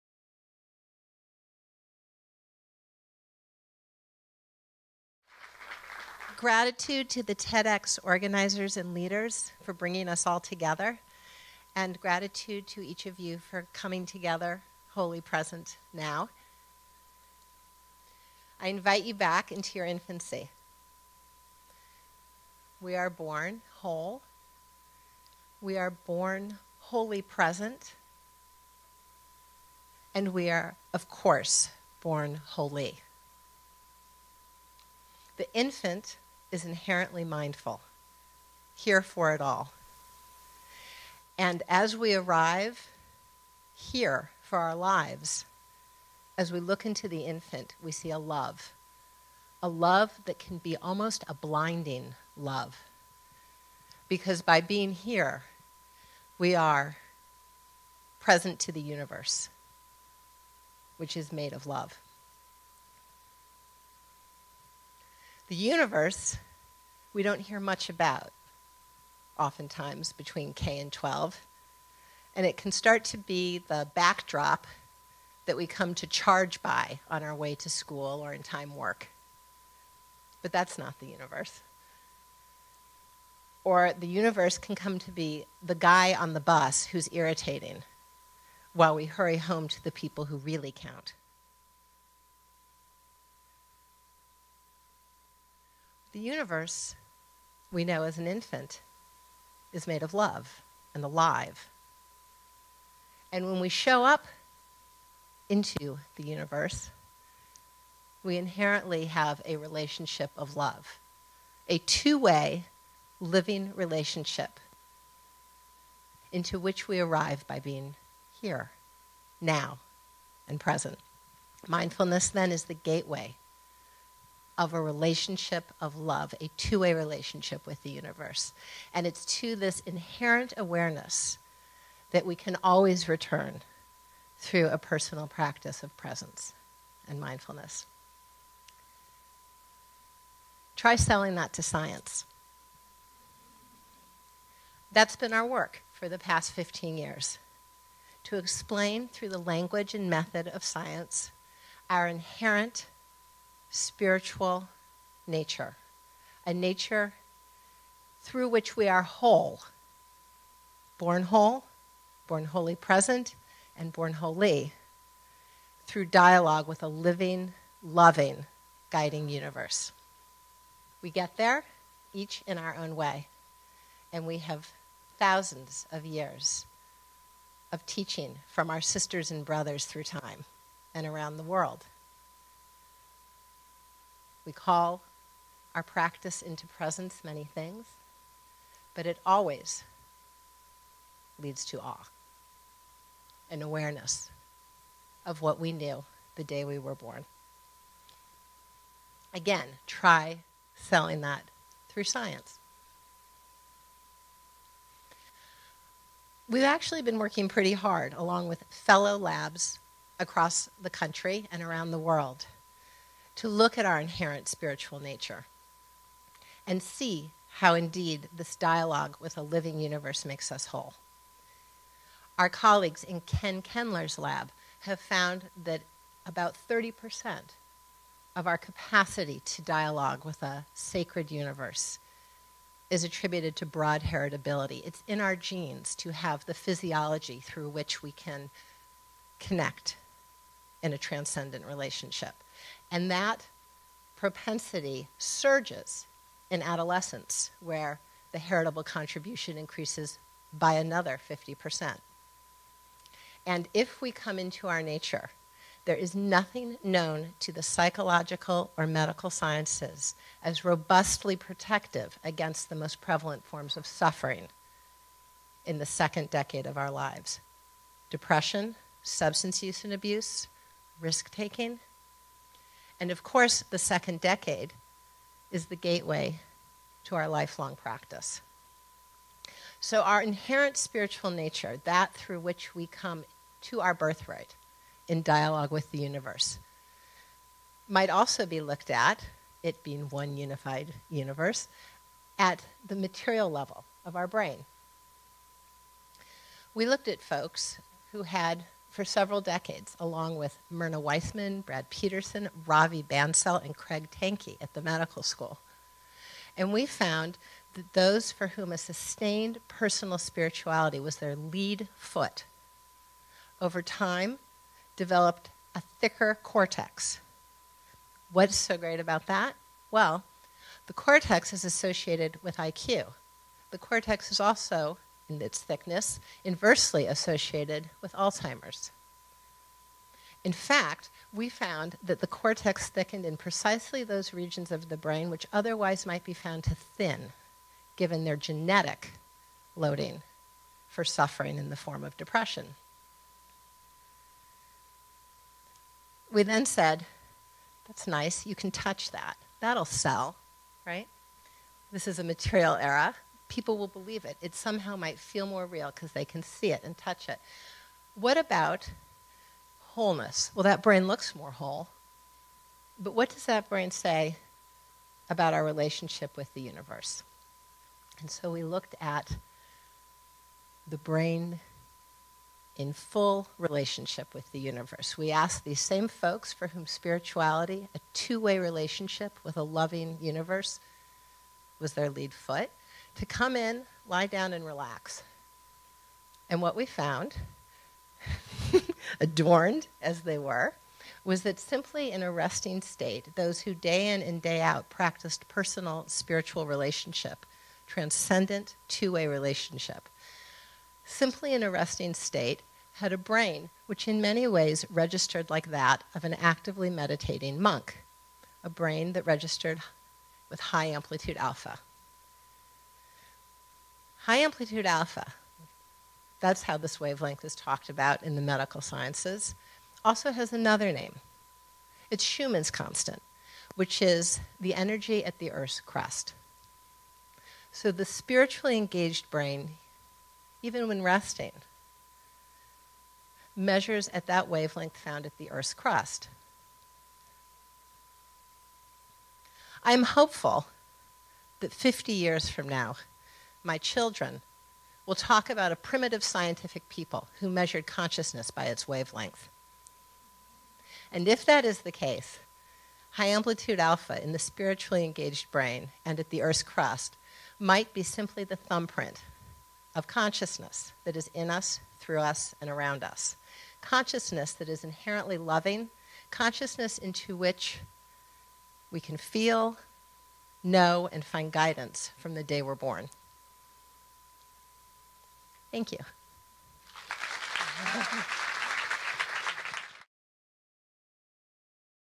Lisa Miller at TEDxTeachersCollege
In this spiritually awakening talk, Lisa Miller invites listeners to assume a child-like curiosity for the universe. Explaining that mindfulness is a metaphorical gateway to appreciating one's surroundings and being appreciated by one's surroundings, Miller encourages listeners to develop a connection between the self and a spiritual universe. Citing research evidence of the positive effects of mindfulness practice and spiritualism on the brain at a material level, Miller makes a remarkable link between brain science, consciousness, and the universe.

Lisa Miller, Ph.D. is a member of the faculty of Teachers College, Columbia University. As founder of the Spirituality Mind Body Institute, Dr. Miller's research focuses extensively on spirituality, mental health, and wellness.